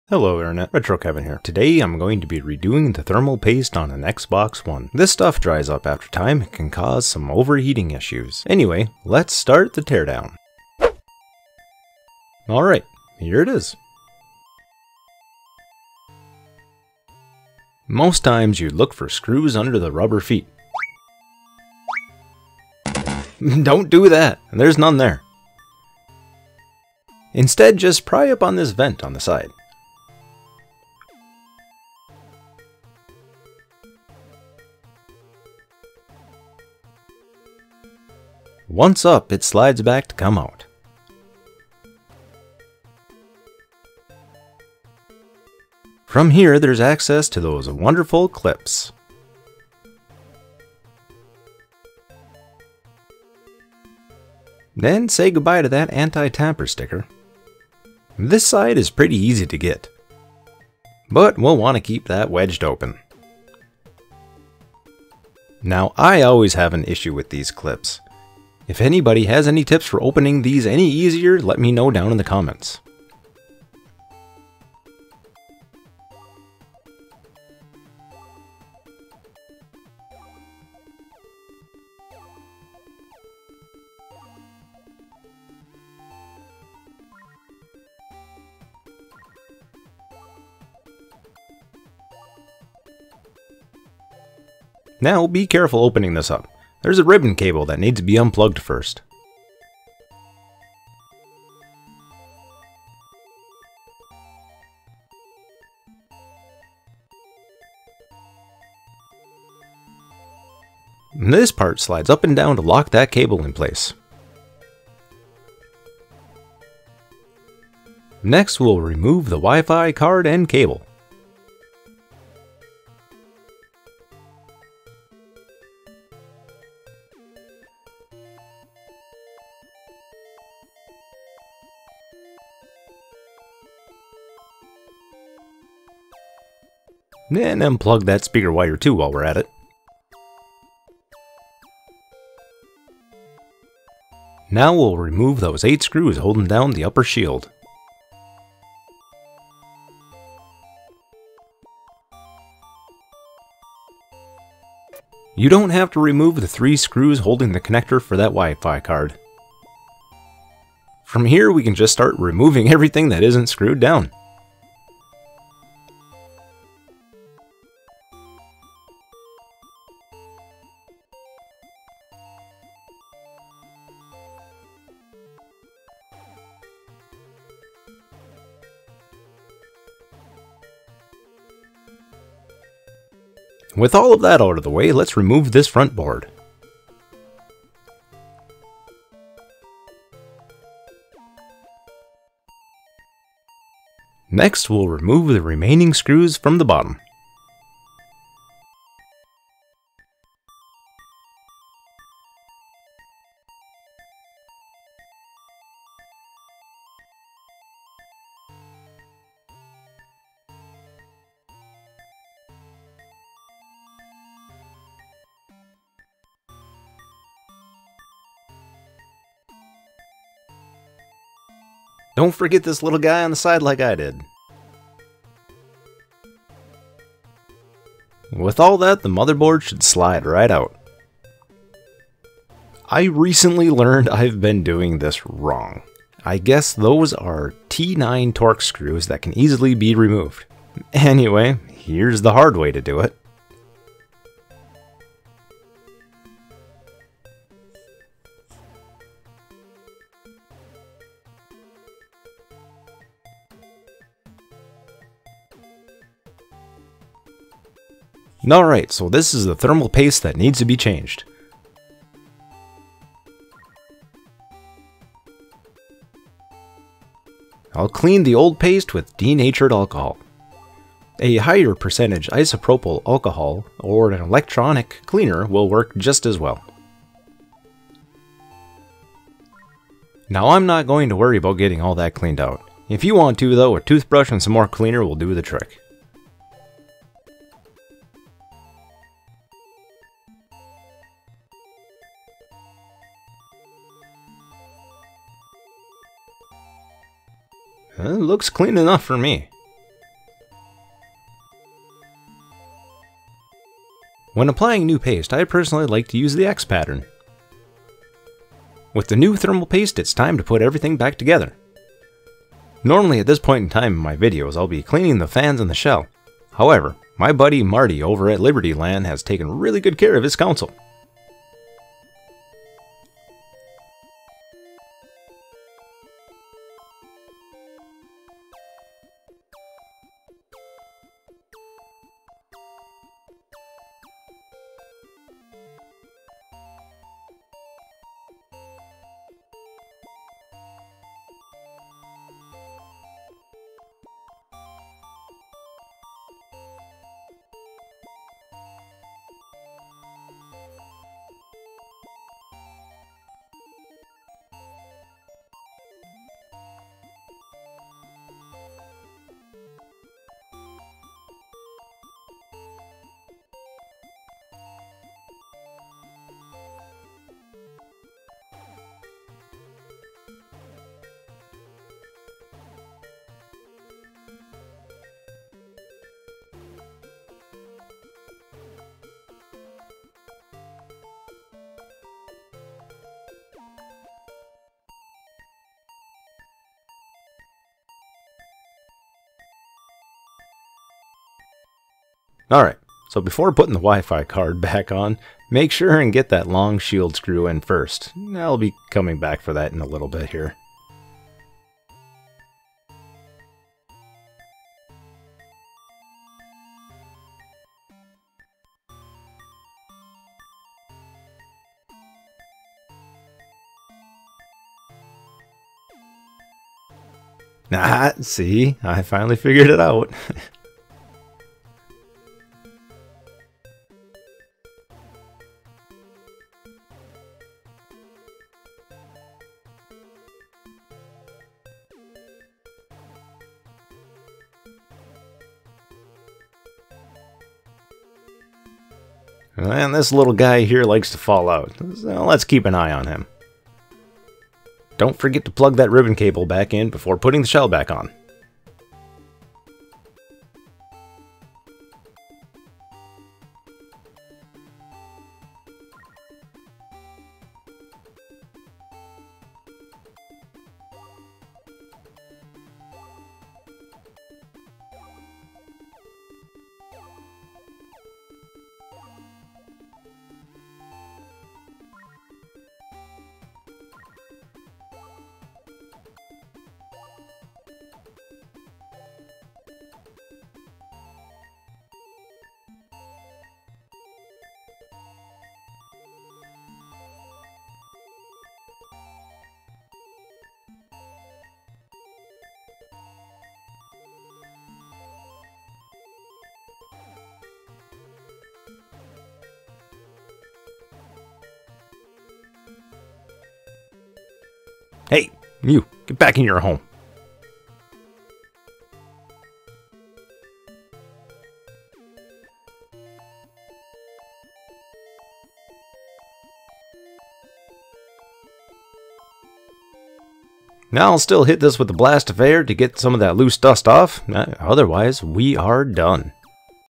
Preventing Xbox One Overheating by replacing the thermal paste
This video I'll be redoing the thermal paste on a Xbox One Halo 5: Guardians Limited Edition.

Doing this can help extend the life of your Xbox.